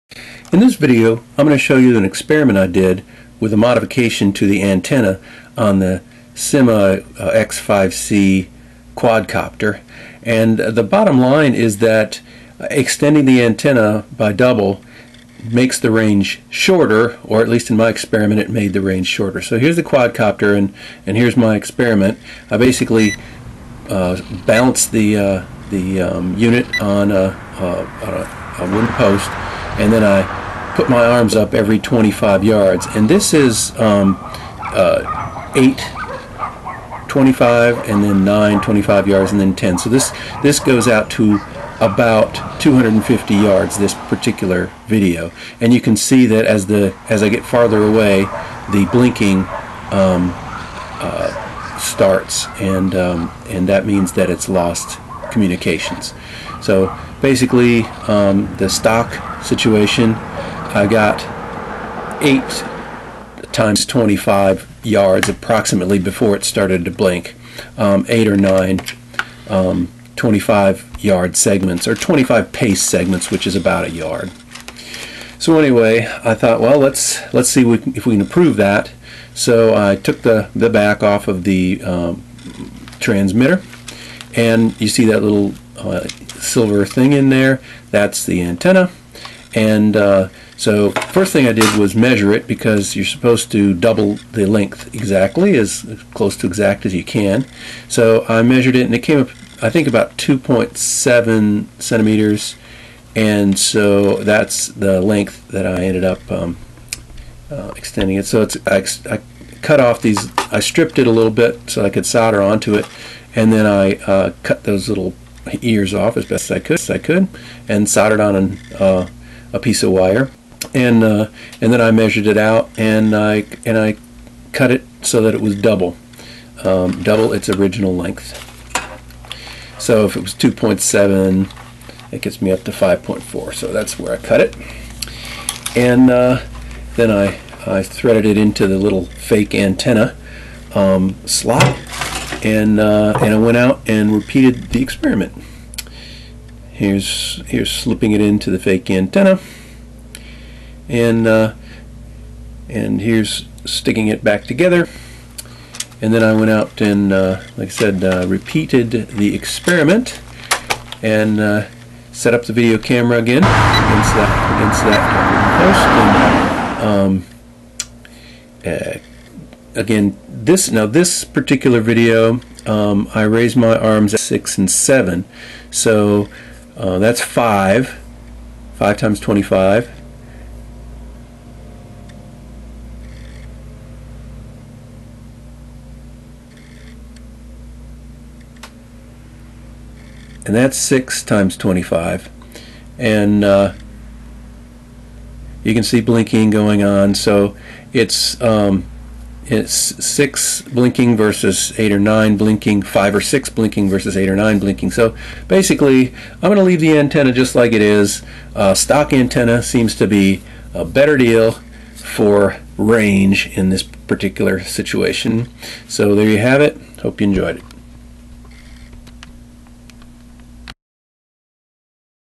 Syma X5C Quadcopter 2.4Ghz Antenna Experiment
After seeing the lights blink while flying (signaling loss of signal), I thought I'd look for hack to increase the range, and I found some that suggested doubling the length of the 2.4Ghz antenna would "double the range".  I figured it would help, but I didn't think it would double the range, so I did this little experiment and determined it REDUCED the range by about 40%!!